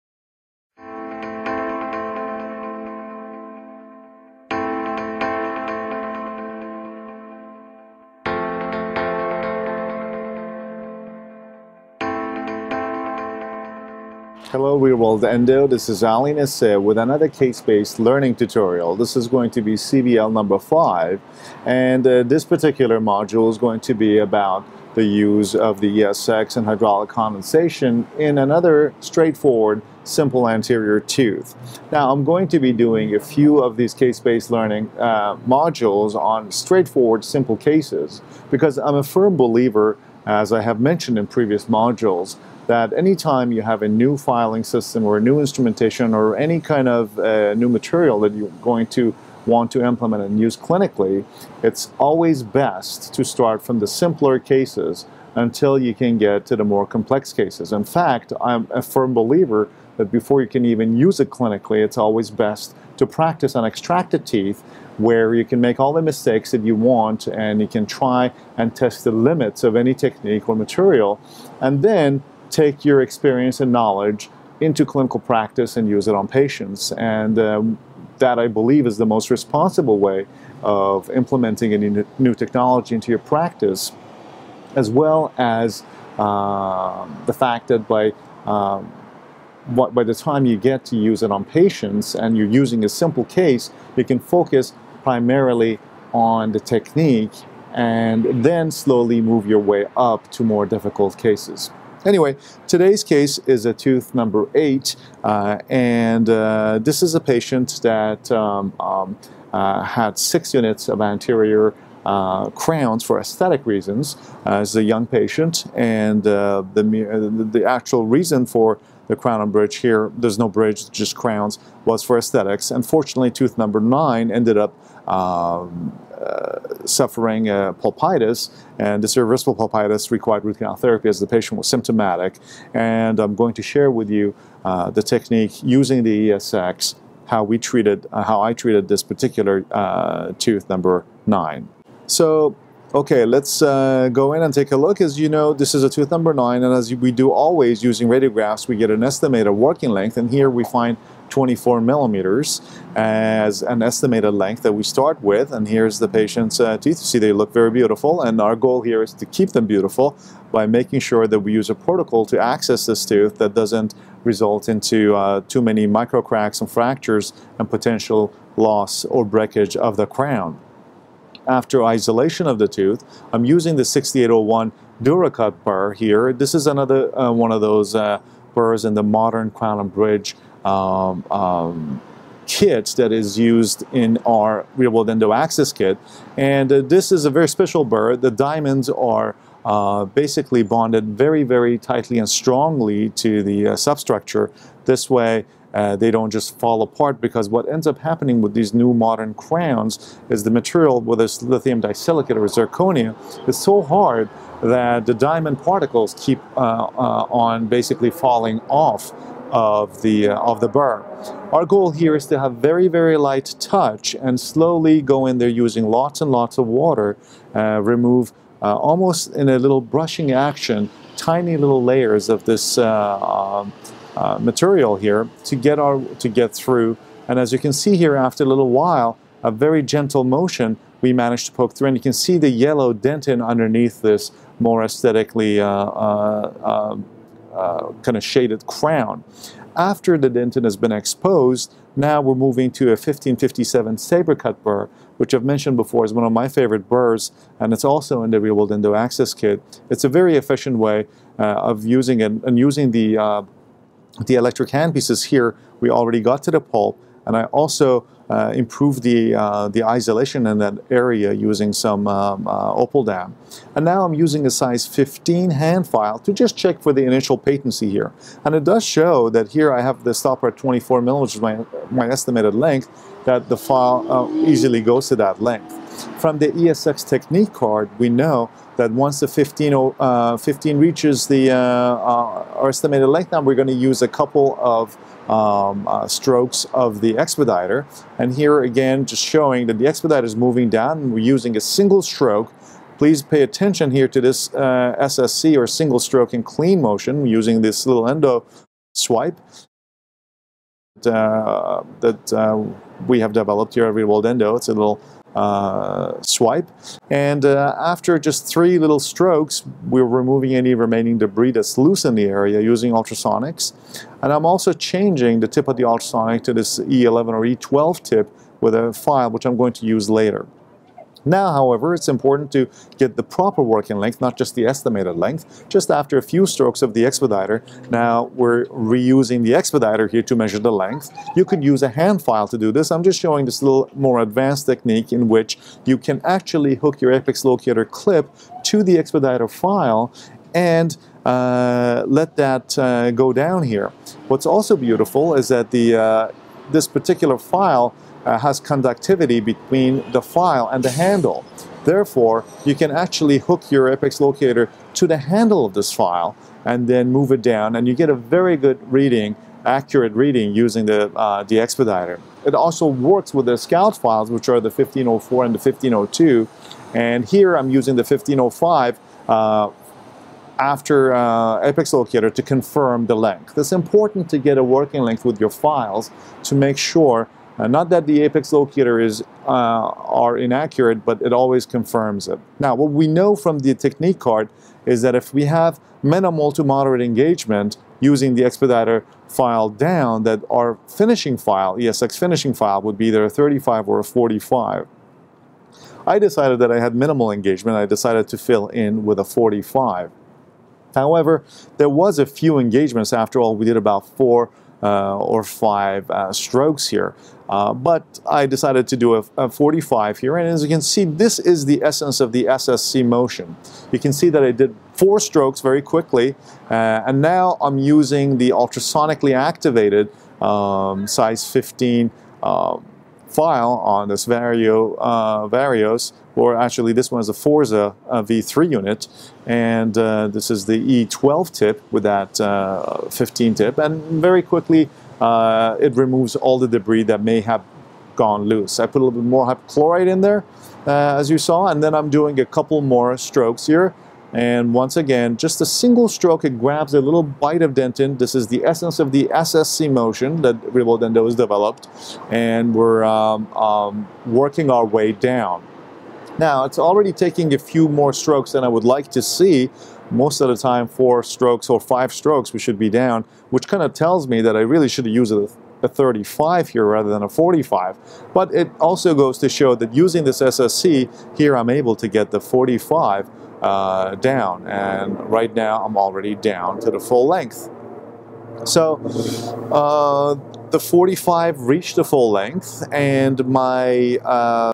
CBL 5 Anterior Tooth ESX Basic Technique
This Case Based Learning (CBL) #5 is an anterior tooth #9 treated with ESX Basic Instrumentation and Hydraulic Condensation